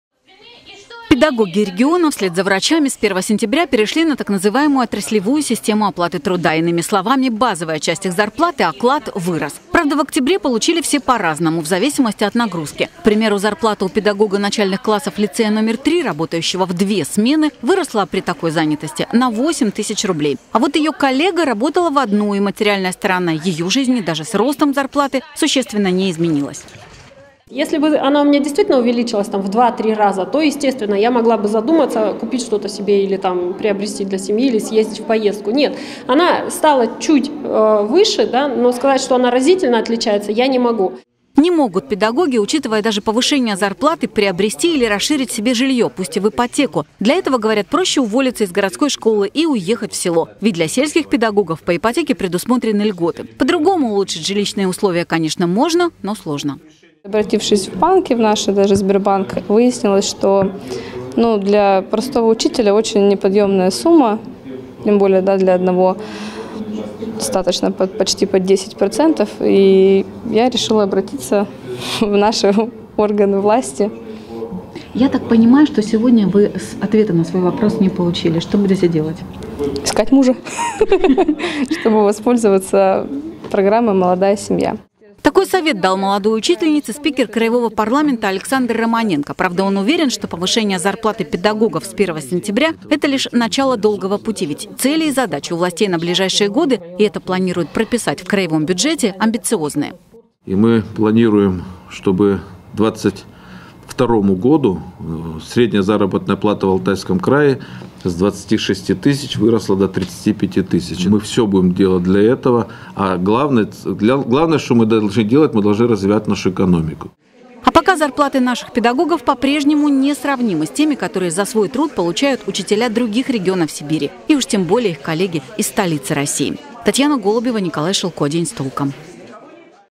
Довольны ли учителя зарплатой, начисленной по новой системе?
В  сентябре педагоги края получили первую зарплату, начисленную по новой системе. Часть оклада теперь значительно больше. Но реально ли доход учителей вырос? Ответ на вопрос пыталась узнать наша съемочная группа вместе с председателем АКЗС Александром Романенко.